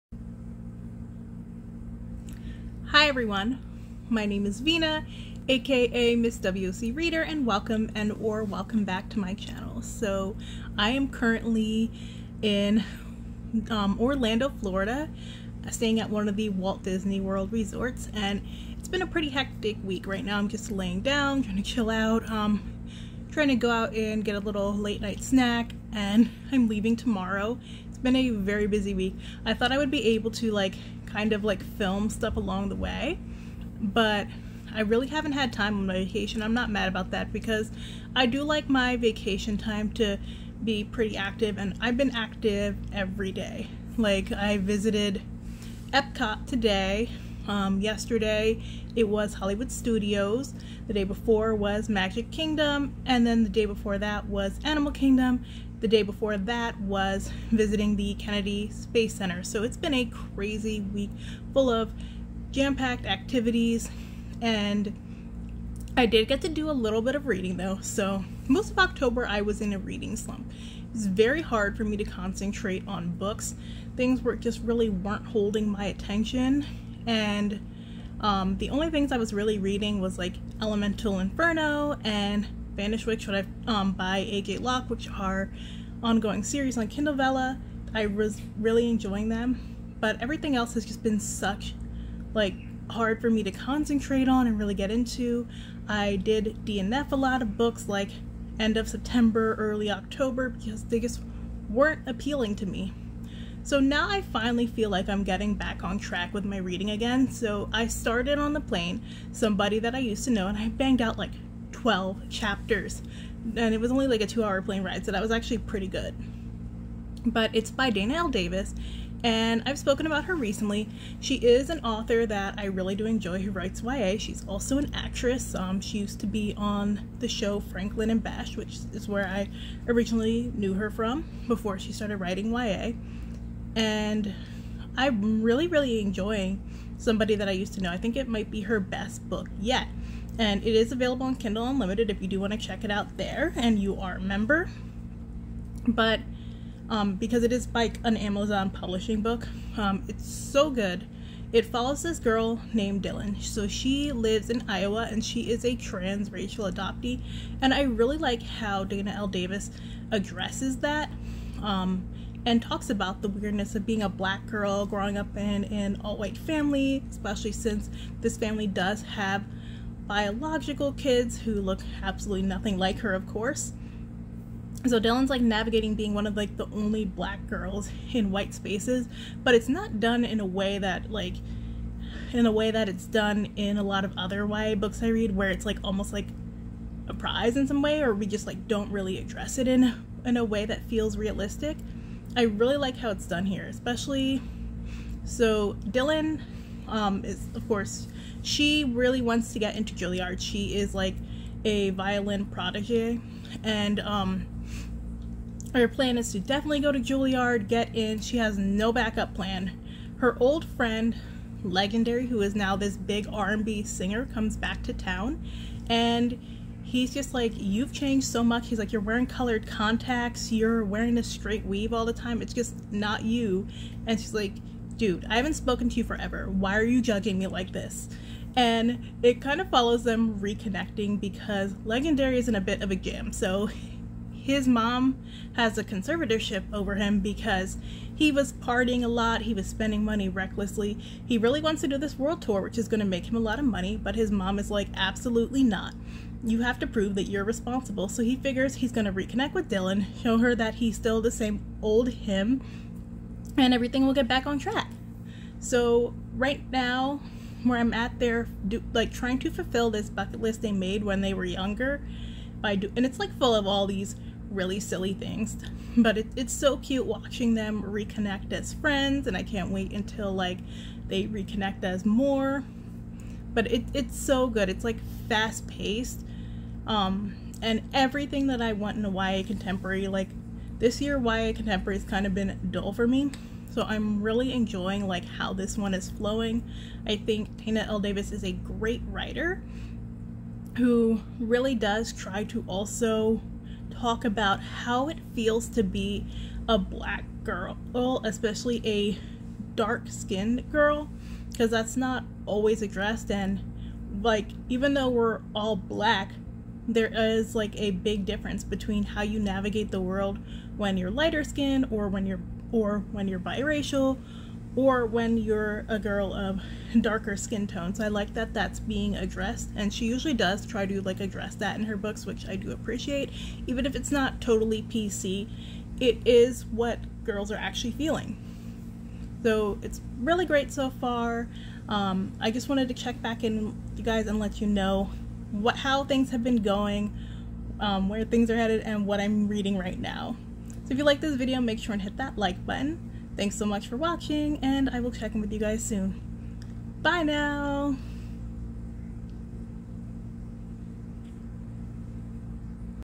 November Check In
How my November is going so far and what I'm reading